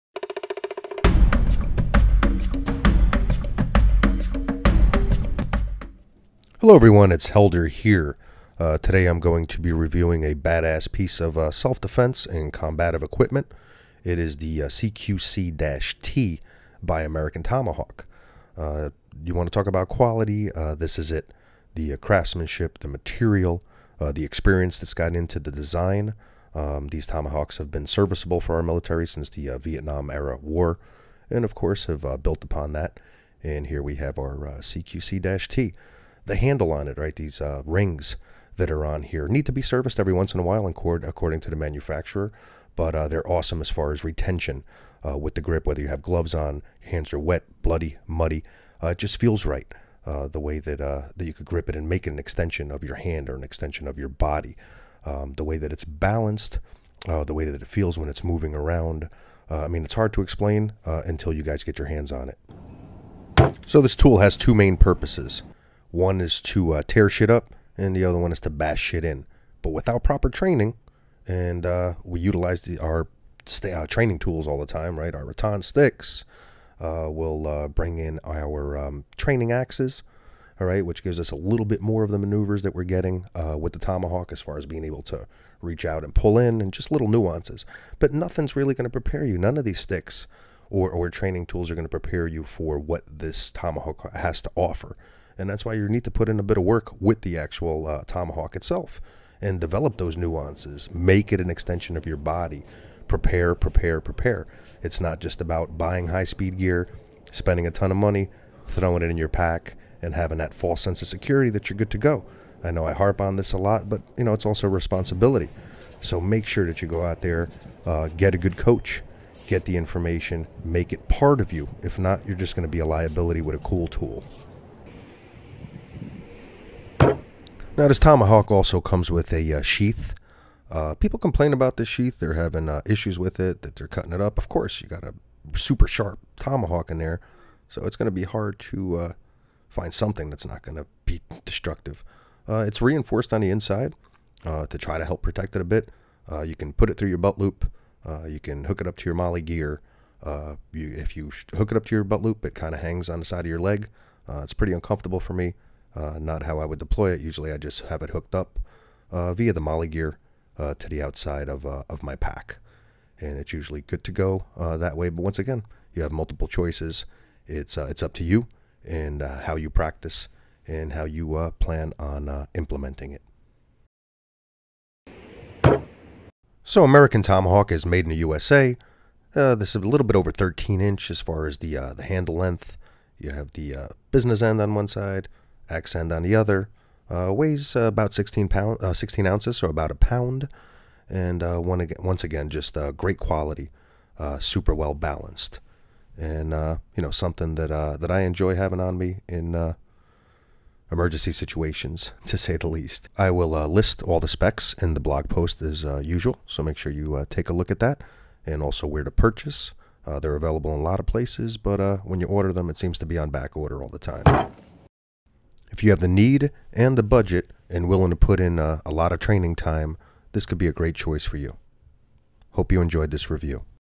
Review of the CQC-T American Tomahawk Tactical Combat Tool
- Coach Helder reviews the American Tomahawk CQC-T and its various applications during self-defense scenarios.